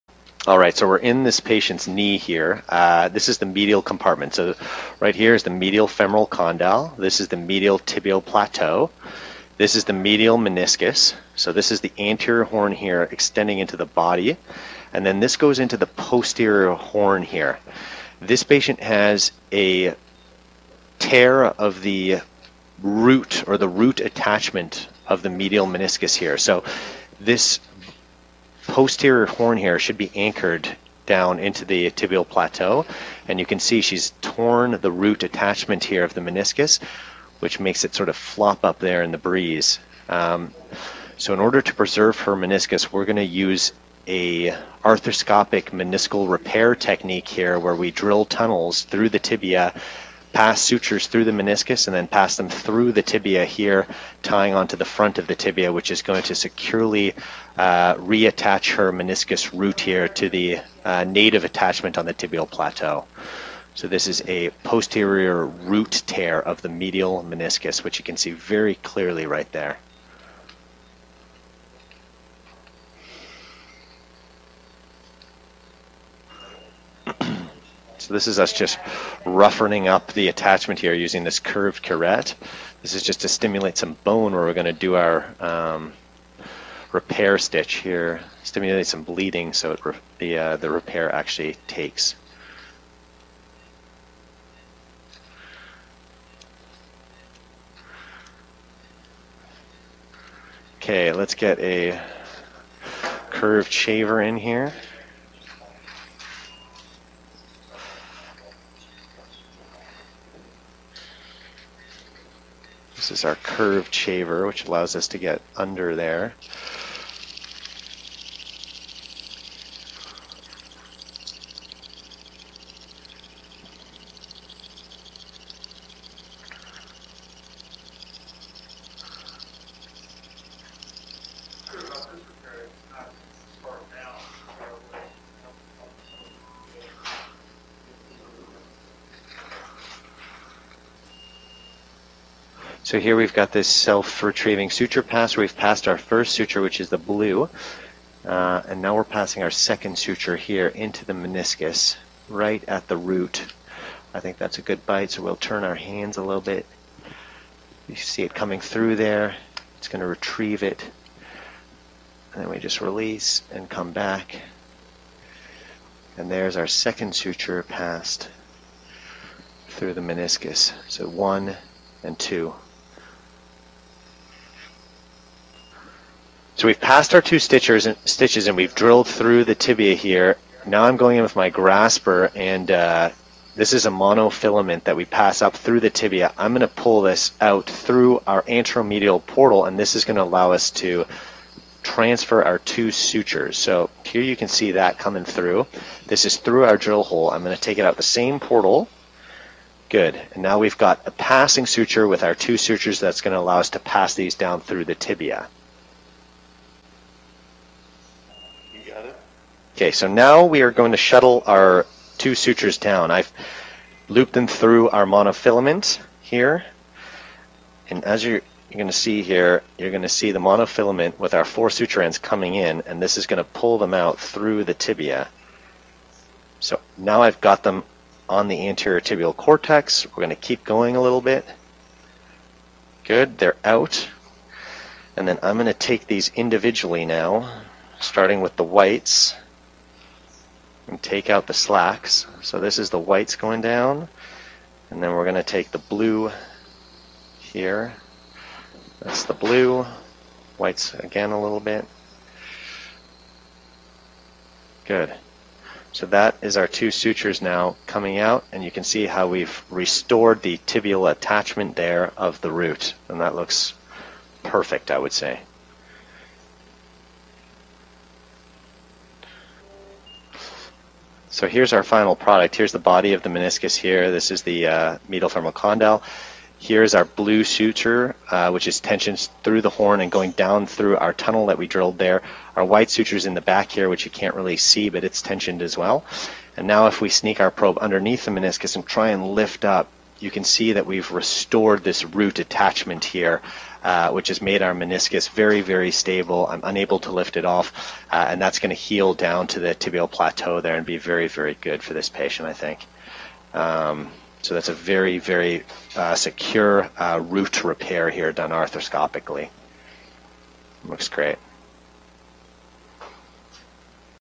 Knee Arthroscopy: Medial Meniscal Root Repair
This video illustrates our technique used for a meniscal root repair of the knee. The patient is a 40 year old who has sustained a tear of the root attachment of the medial meniscus. Two sutures are passed through the meniscal root tissue, retrieved through a tunnel drilled at the native attachment of the root, and tensioned and tied over the anterior tibia. This appropriated secures the meniscal tissue at its native root attachment.